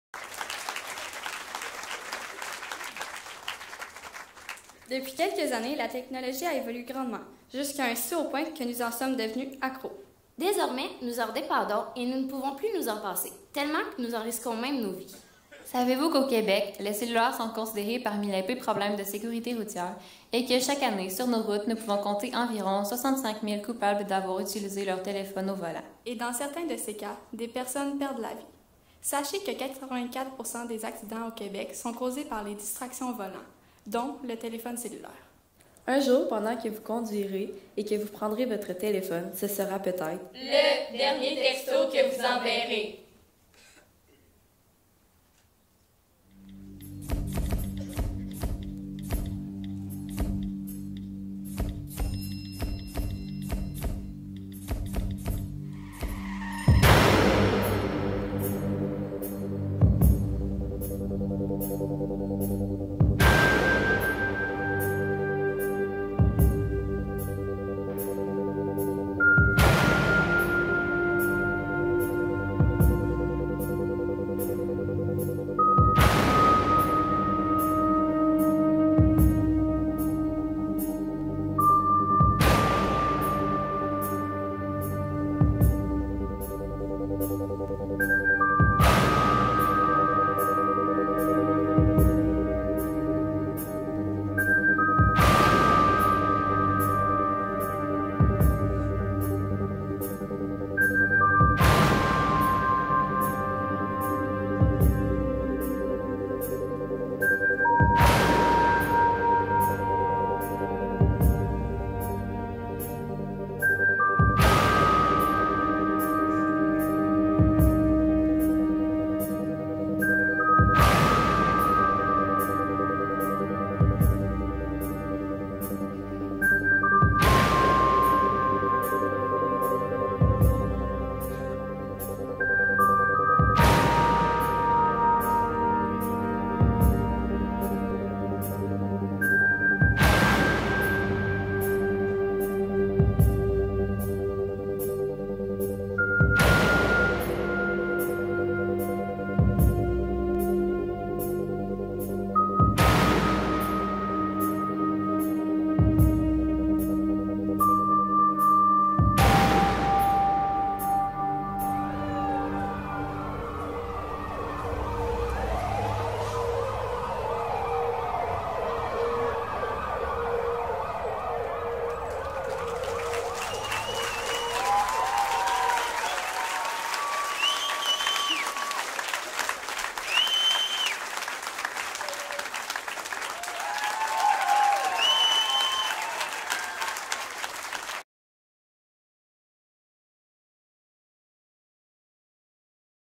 |Danse| secondaire en spectacle 2017: le dernier texto
Salut tout le monde!! Donc cette semaine on vous revient avec la video de notre performance à secondaire en spectacle au finale local!